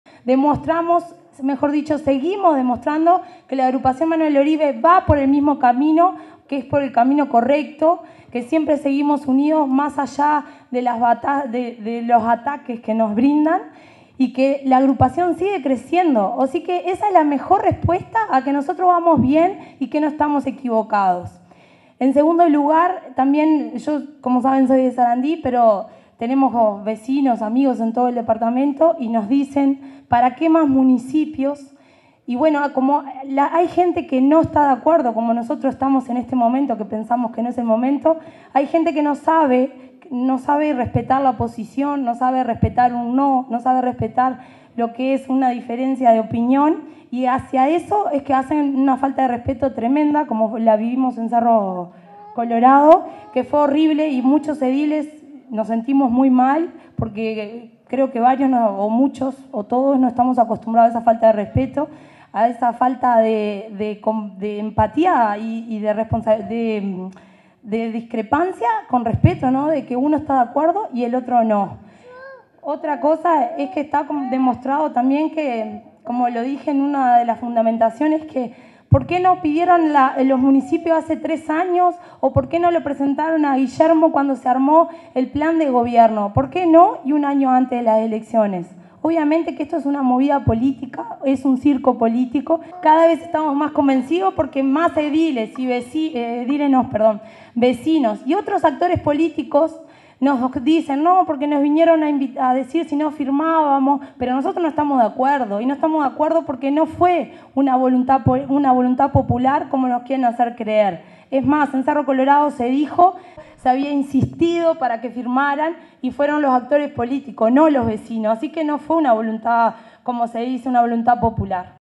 Lucia Pereyra
“Seguimos demostrando que la Agrupación Manuel Oribe va por el camino correcto y unidos más allá de los ataques”, indicó la edil Lucía Pereyra.
“Hay gente que no sabe respetar un no”, señaló, asegurando que fueron víctima de falta de respeto en Cerro Colorado.